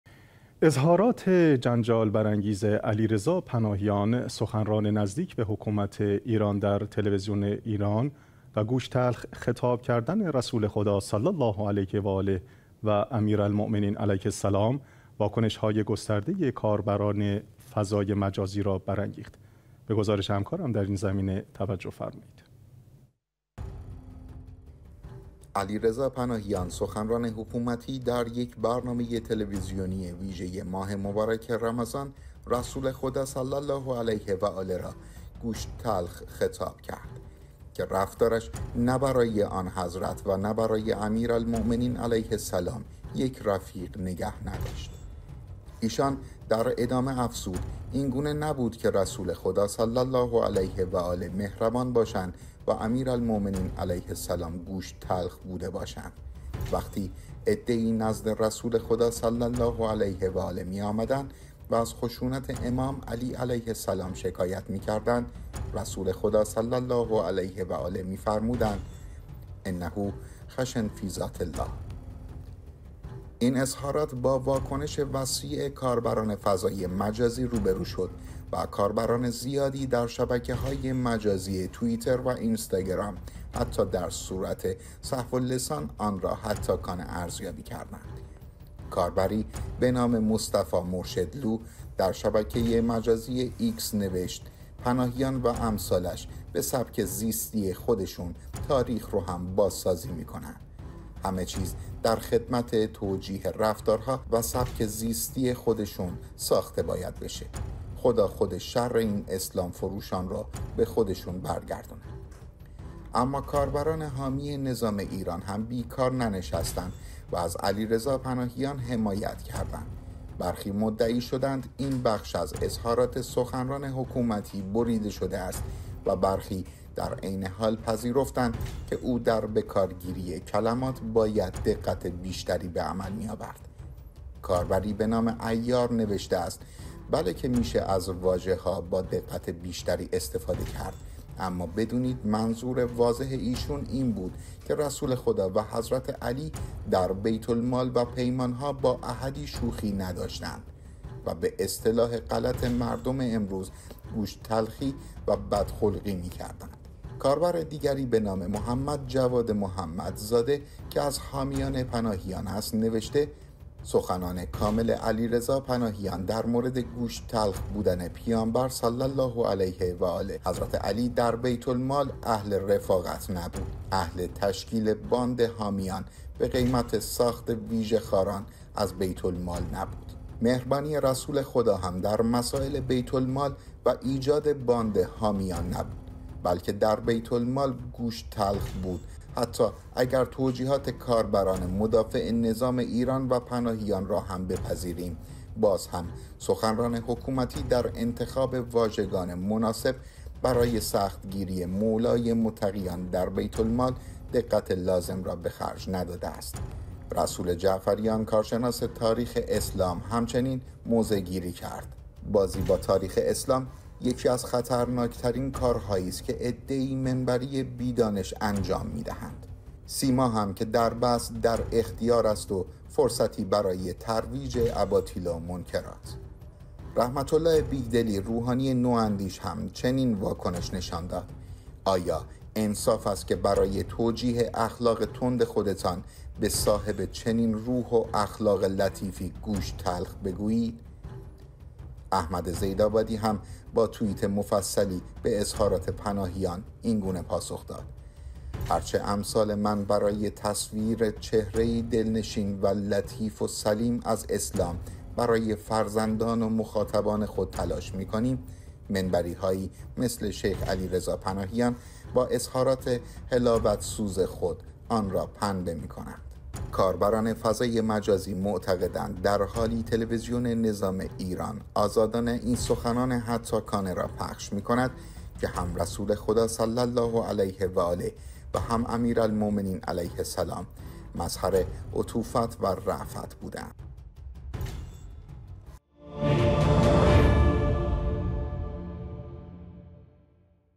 پناهیان و اظهارات جنجال برانگیز در تلویزیون ایران، 👈 👈 به گزارش دراین زمینه توجه فرمایید
اظهارات جنجال برانگیز علیرضا پناهیان سخنران نزدیک به حکومت ایران در تلویزیون ایران و گوشت تلخ خطاب کردن رسول خدا صلی الله علیه و آله و امیر المومنین علیه السلام واکنش های گسترده‌ی کاربران فضای مجازی را برانگیخت.

علیرضا پناهیان سخنران حکومتی در یک برنامه تلویزیونی ویژه ماه مبارک رمضان رسول خدا صلی الله علیه و آله را  "گوشت تلخ" خطاب کرد که رفتارش نه برای آن حضرت و نه برای امیر المومنین علیه السلام یک رفیق نگه نداشت.
ایشان در ادامه افزود: اینگونه نبود که رسول خدا صلی الله علیه و آله مهربان باشند و امیر المومنین  علیه السلام گوشت تلخ بوده باشند ، وقتی عده ای نزد رسول خدا صلی الله علیه و آله می‌آمدند و از خشونت امام علی علیه السلام شکایت می کردند رسول خدا صلی الله علیه و آله می فرمودند: لاتشکوا علیا انه خشن فی ذات الله.
این اظهارات با واکنش وسیع کاربران فضای مجازی روبرو شد و کاربران زیادی در شبکه های مجازی توییتر و اینستاگرام حتی در صورت سهواللسان آنرا هتاکانه ارزیابی کردند.
کاربری به نام مصطفی مرشد لو در شبکه مجازی ایکس نوشت: پناهیان و امثالش به سبک زیستی خودشون تاریخ رو هم بازسازی می‌کنن. همه چیز در خدمت توجیه رفتارها و سبک زیستی خودشون برساخته باید بشه. خدا خودش شر این اسلام‌فروشان رو به خودشون برگردونه @morshedloo
اما کاربران حامی نظام ایران هم بیکار ننشستند و از علیرضا پناهیان حمایت کردند. برخی مدعی شدند این بخش از اظهارات سخنران حکومتی بریده شده است و برخی در عین حال پذیرفتند که او در بکارگیری کلمات باید دقت بیشتری بعمل می آورد.
کاربری به نام عیار نوشته است: بله که میشه از واژه‌ها با دقت بیشتری استفاده کرد اما بدونید منظور واضح ایشون این بود که رسول خدا و حضرت علی در بیت‌المال و پیمان‌ها با احدی شوخی نداشتن و به اصطلاح غلط مردمِ امروز، گوشت تلخی و بد خلقی می‌کردن!
کاربر دیگری به نام محمد جواد محمد زاده که از حامیان پناهیان است نوشته : سخنان کامل علیرضا پناهیان در مورد گوشت‌تلخ بودن پیامبر(ص): حضرت علی در بیت‌المال اهل رفاقت نبود. اهل تشکیل باندِ حامیان به قیمت ساخت ویژه‌خواران از بیت‌المال نبود.
مهربانیِ رسول خدا هم در مسائل بیت‌المال و ایجاد باند حامیان نبود. بلکه در بیت‌المال گوشت‌تلخ بود
حتی اگر توجیهات کاربران مدافع نظام ایران و پناهیان را هم بپذیریم باز هم سخنران حکومتی در انتخاب واژگان مناسب برای سخت گیری مولای متقیان در بیت المال دقت لازم را بخرج نداده است.

رسول جعفریان کارشناس تاریخ اسلام هم چنین موضع گیری کرد:
بازی با تاریخ اسلام، یکی از خطرناک‌ترین کار‌هایی است که عده‌ای منبری بی دانش انجام می‌دهند
سیما هم که دربست در اختیار است و فرصتی برای ترویج اباطیل و منکرات
رحمت الله بیگدلی  روحانی نواندیش هم چنین واکنش نشان داد
آیا انصاف است که برای توجیه اخلاق تندِ خودتان به صاحب چنین روح و اخلاق لطیفی گوشت‌تلخ بگویید؟!
احمد زید آبادی هم با توییت مفصلی به اظهارات پناهیان اینگونه پاسخ داد: هر چه امثال من برای تصویر چهره‌ای دلنشین و لطیف و سلیم از اسلام برای فرزندان و مخاطبان خود تلاش می‌کنیم، منبری‌هایی مثل شیخ علی‌رضا پناهیان با اظهارات حلاوت‌سوز خود آن را پنبه می‌کنند!
کاربران فضای مجازی معتقدند در حالی تلویزیون نظام ایران آزادانه این سخنان هتاکانه را پخش می کند که هم رسول خدا صلی الله علیه و آله و هم امیر المومنین علیه السلام مظهر عطوفت و رافت بودند.